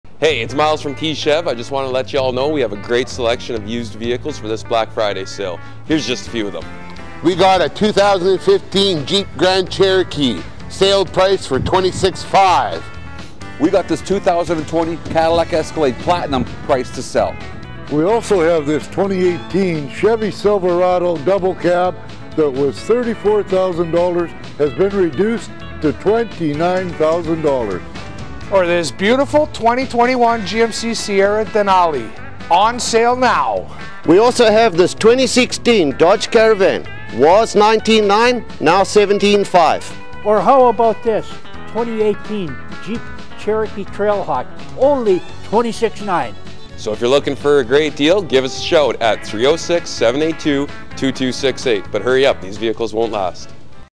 Black Friday Sale
We've got some great deals for our used vehicle black Friday sale! and come in early for the best selection!

At Key Chevrolet Cadillac Buick GMC we will always go above and beyond for our customers. Stop in to the dealership, located at 441 Broadway Ave. in Yorkton, to learn more.